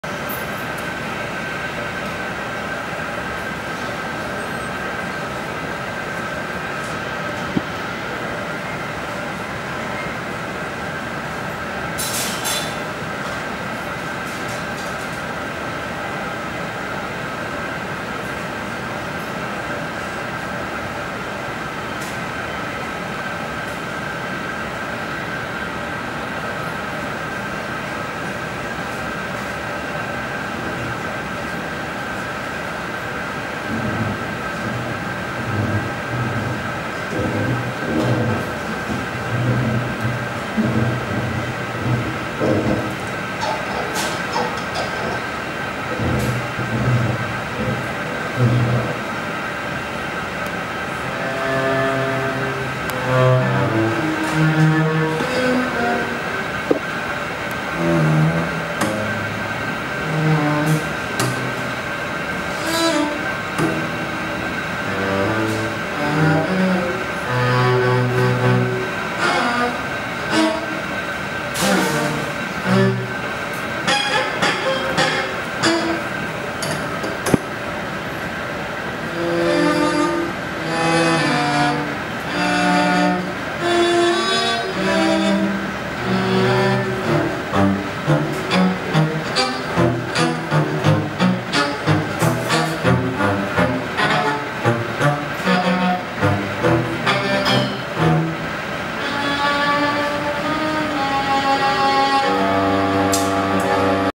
Space Between Dreams
"Space Between Dreams". Juliana Ponguta Forero (dancer), Gary Rouzer (musician) Chrysler Museum, Glass Studio, Norfolk, Virginia- August 2016. Conceived by Nancy Havlik & Rui Sasaki.